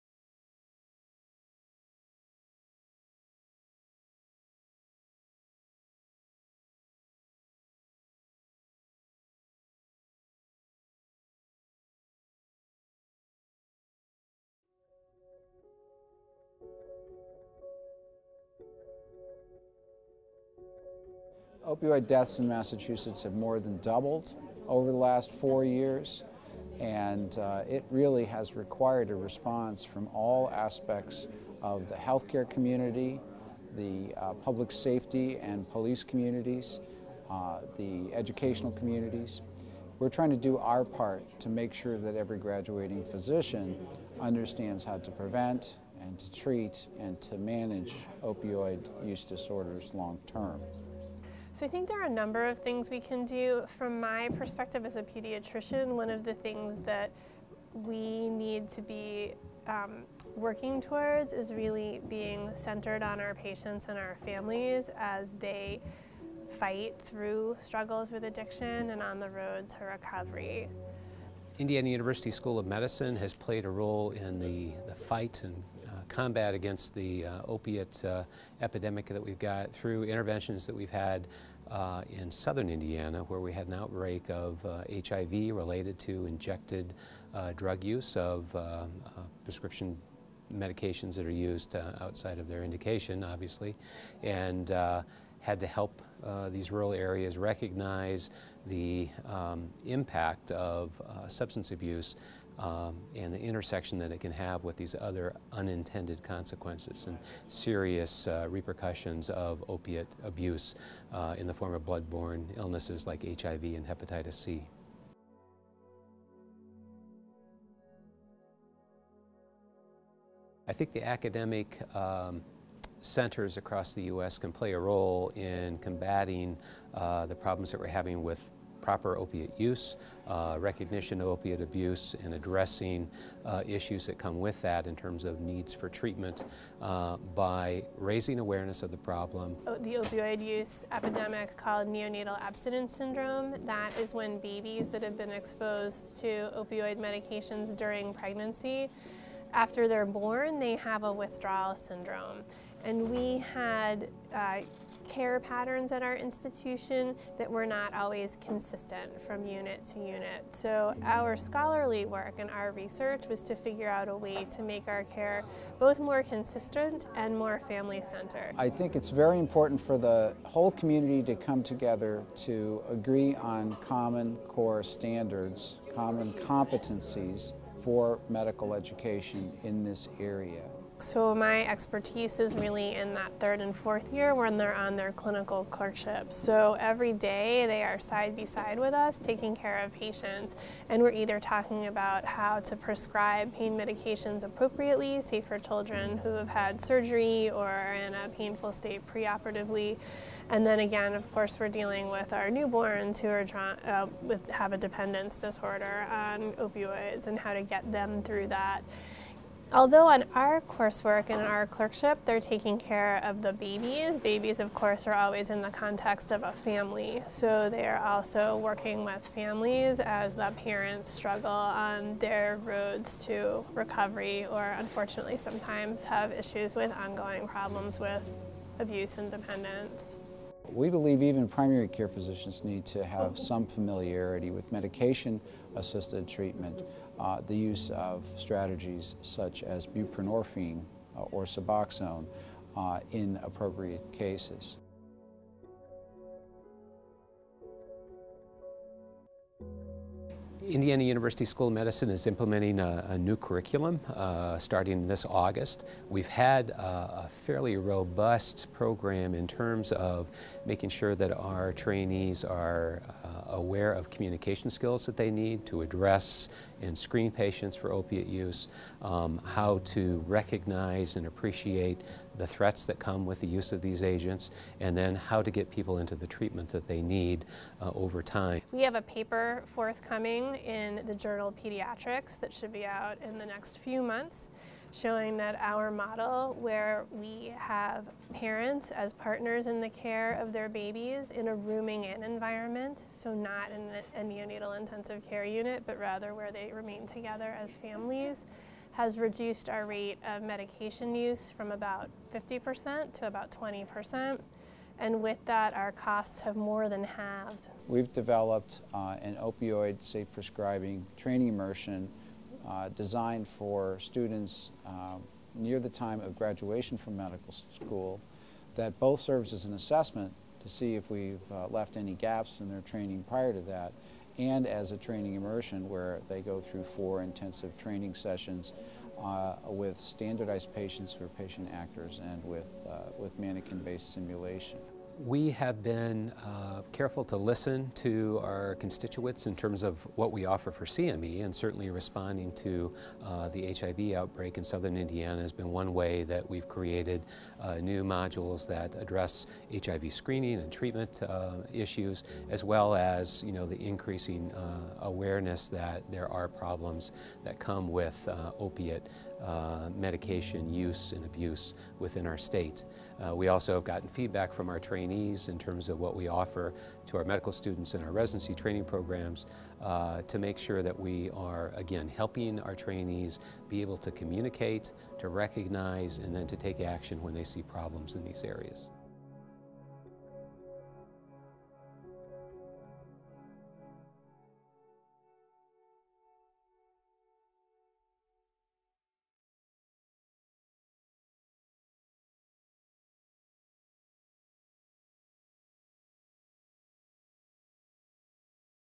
How Medical Schools and Teaching Hospitals are Addressing the Opioid Epidemic
Part of the mission of medical schools and teaching hospitals is to respond to community and public health crises through innovations in medical education, patient care, and research.

“The response to the opioid epidemic is still evolving, just as medicine and our education evolves,” said Atul Grover MD, PhD, chief public policy officer at the Association of American Medical Colleges (AAMC) and moderator of a January 28 Capitol Hill Briefing titled "How Medical Schools and Teaching Hospitals are Addressing the Opioid Epidemic.” The briefing, hosted by the AAMC and the Congressional Academic Medicine Caucus, focused on the role of academic medicine in the response to the opioid epidemic and to other related public health issues.

Panel participants included Terence Flotte, MD, dean of the School of Medicine at the University of Massachusetts (UMass) Medical School; Bradley Allen, MD, PhD, FACP, FIDSA, senior associate dean for medical school education at the Indiana University School of Medicine; and Alison Holmes, MD, MPH, MSc, associate professor of pediatrics and of The Dartmouth Institute, at the Geisel School of Medicine at Dartmouth. Panelists described how training at medical schools and teaching hospitals exposes students and residents to firsthand experiences with innovative clinical care and research efforts aimed at tackling the most pressing community health challenges.